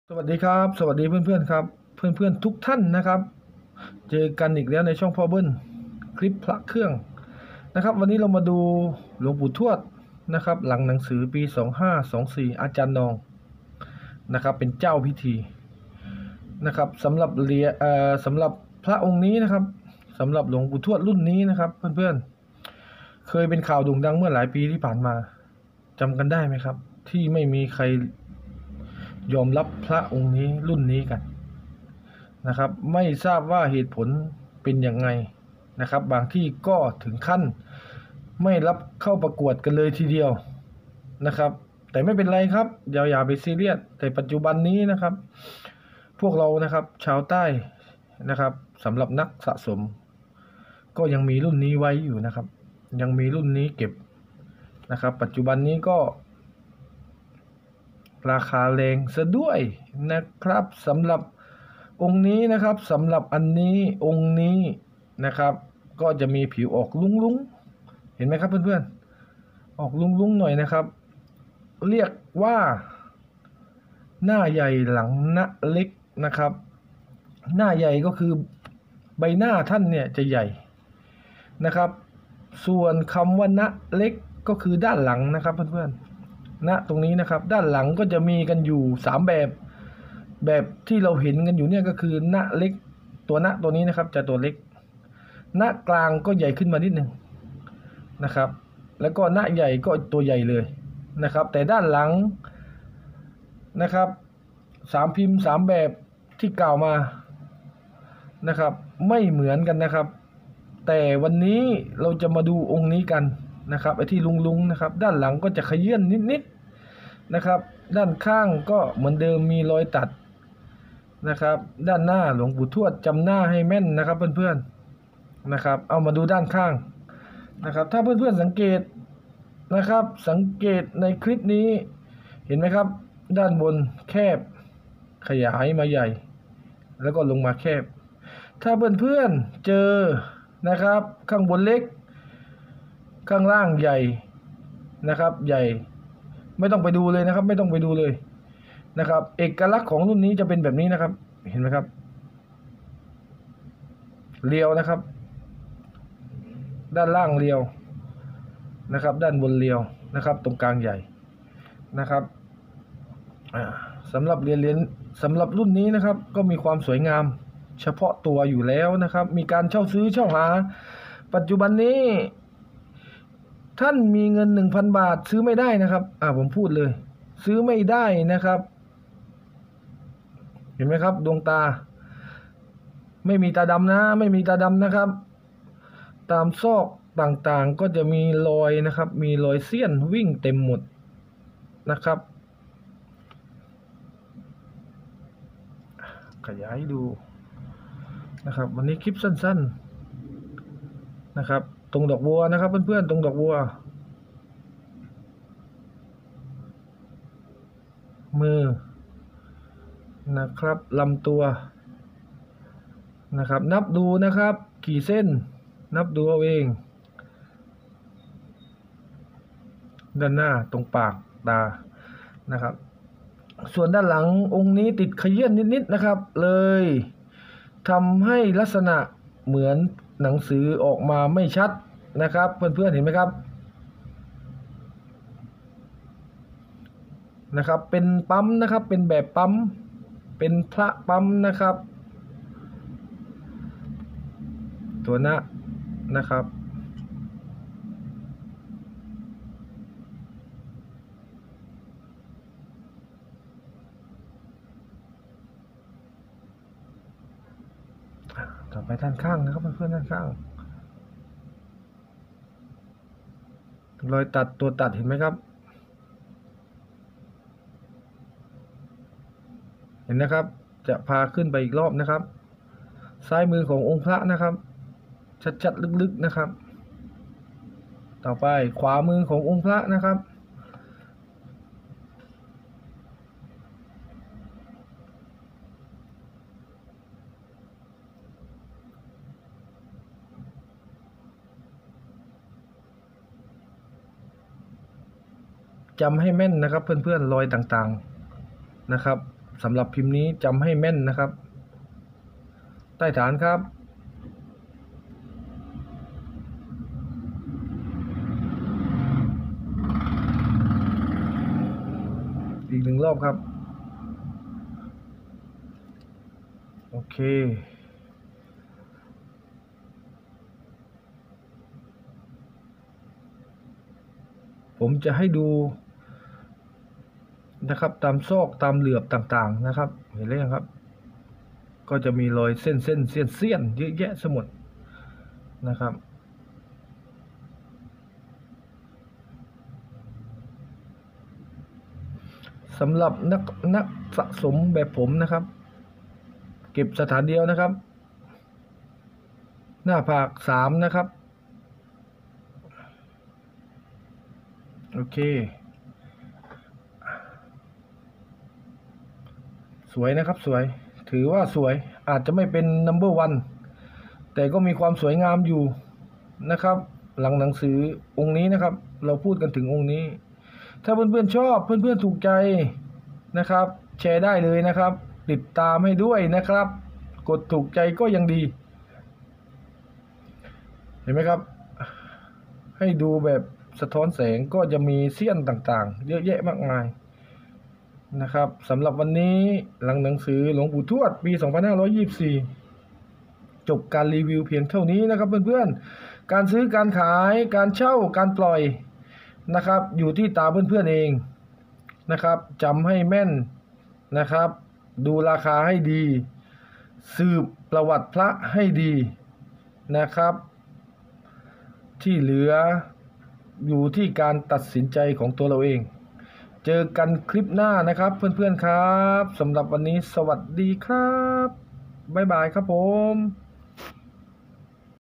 หลวงพ่อทวด หลังหนังสือปี2524วัดช้างให้
หลวงพ่อทวดหลังหนังสือ ปี2524 วัดช้างให้ พิมพ์หน้าใหญ่ หลังนะเล็ก รุ่นนี้อาจารย์นอง เกจิดังแห่งภาคใต้ร่วมปลุกเสก เป็นอีกรุ่นที่ได้รับควาทนิยม ในปี2563 กะแสแรงไม่มีตก